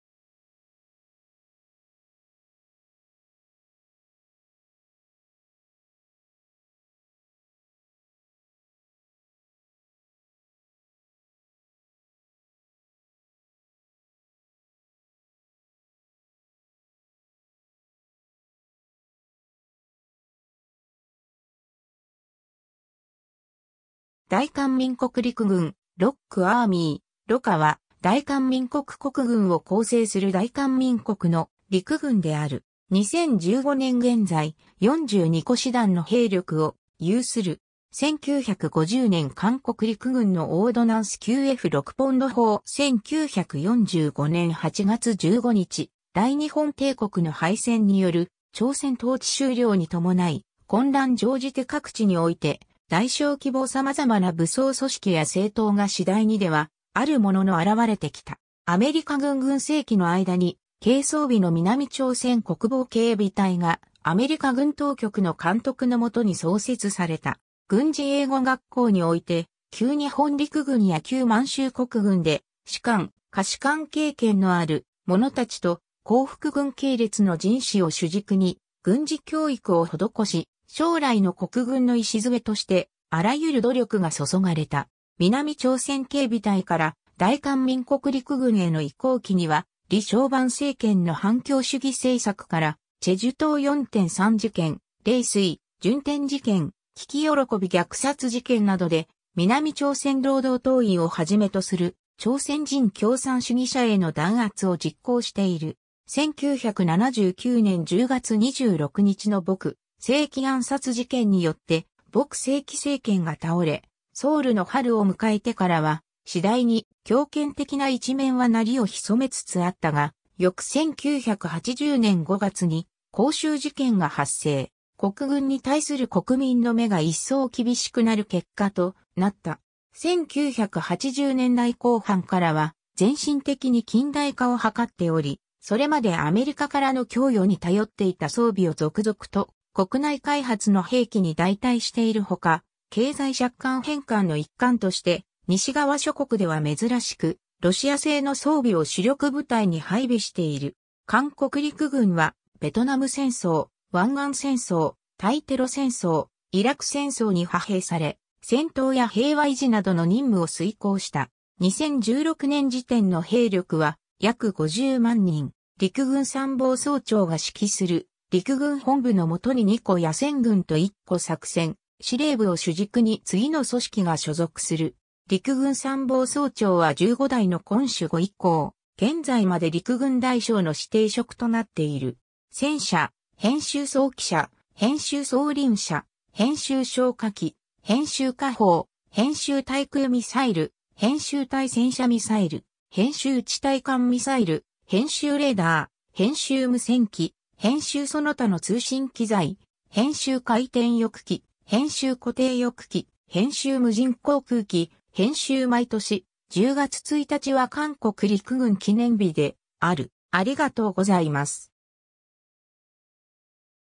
大韓民国陸軍
大韓民国陸軍 （だいかんみんこくりくぐん、朝鮮語: 대한민국 육군）、ROK Army、ROKA） は、大韓民国国軍を構成する大韓民国の陸軍である。
 2015年現在、42個師団（常備師団 22個、予備師団 20個、常備軍 約50万人、予備軍 約320万人）の兵力を有する。
 1950年韓国陸軍のオードナンス QF 6ポンド砲 1945年8月15日、大日本帝国の敗戦による朝鮮統治終了にともない、混乱乗じて各地において大小規模さまざまな武装組織や政党が次第にではあるものの現れてきた。
 アメリカ軍軍政期の間に軽装備の南朝鮮国防警備隊（旧日本軍の置き土産でもある三八式歩兵銃や九九式短小銃、中には竹槍で一時を凌いだ部隊もあり、制服や靴は員数分も満足に無く日々の食事にも困ることもあった。しかし当時の韓国国家警察はカービン銃をはじめ、戦車以外の必要な装備はアメリカ軍から与えられていた）がアメリカ軍当局の監督の元に創設された。
軍事英語学校（後の警備士官学校/陸軍士官学校）において旧日本陸軍や旧満州国軍で士官・下士官経験のある者たちと光復軍系列の人士を主軸に軍事教育を施し、将来の国軍の礎としてあらゆる努力が注がれた。
 南朝鮮警備隊から大韓民国陸軍への移行期には、李承晩政権の反共主義政策から、済州島四・三事件、麗水・順天事件、聞慶虐殺事件などで南朝鮮労働党員をはじめとする朝鮮人共産主義者への弾圧を実行している。
 1979年10月26日の朴正煕暗殺事件によって朴正煕政権が倒れ、「ソウルの春」を迎えてからは次第に（あるいは表面的には）強権的な一面は鳴りを潜めつつあったが、翌1980年5月に光州事件が発生。
国軍に対する国民の目が一層厳しくなる結果となった。
 1980年代後半からは、漸進的に近代化を図っており、それまでアメリカからの供与に頼っていた装備を続々と国内開発の兵器に代替しているほか、経済借款返還の一環として、西側諸国では珍しくロシア製の装備を主力部隊に配備している。
 韓国陸軍は、ベトナム戦争、湾岸戦争、対テロ戦争、イラク戦争に派兵され、戦闘や平和維持などの任務を遂行した。
 2016年時点の兵力は約500,000人。
 陸軍参謀総長が指揮する陸軍本部のもとに2個野戦軍と1個作戦司令部を主軸に次の組織が所属する。
 陸軍参謀総長（육군참모총장）は15代の金鐘五以降現在まで陸軍大将の指定職となっている。
 戦車[編集] 装軌車[編集] 装輪車[編集] 小火器[編集] 火砲[編集] 対空ミサイル[編集] 対戦車ミサイル[編集] 地対艦ミサイル[編集] レーダー[編集] 無線機[編集] その他の通信器材[編集] 回転翼機[編集] 固定翼機[編集] 無人航空機[編集] 毎年、10月1日は韓国陸軍記念日である。